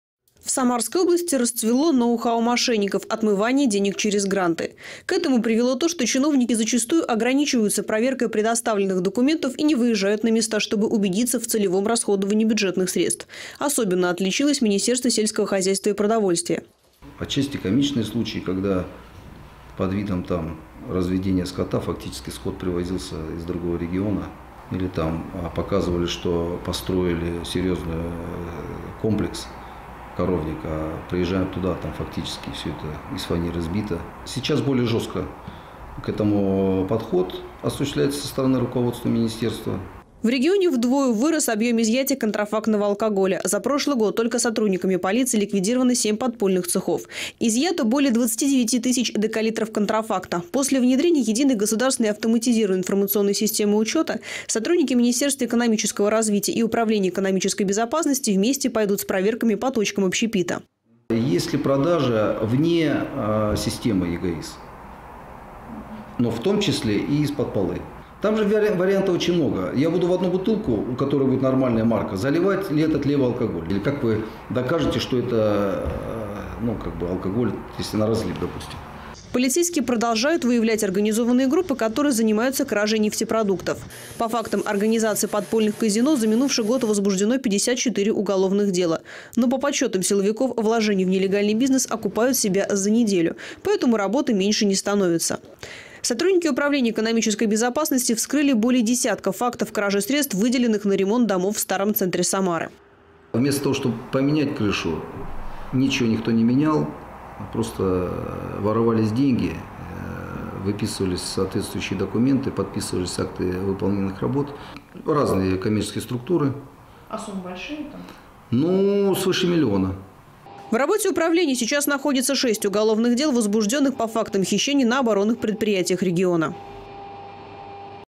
Управлению по борьбе с экономическими преступлениями - 80 лет
В Самарской области в прошлом году зафиксировано около двух тысяч экономических преступлений с общим ущербом для государства на сумму свыше двух миллиардов рублей. Выросло количество фактов хищения бюджетных средств и крупных взяток. Об этом в эксклюзивном интервью корреспондентам Терры рассказал начальник регионального управления по экономической безопасности и противодействию коррупции Михаил Яркин. Сегодня 80-летний юбилей службы. Подробности - далее.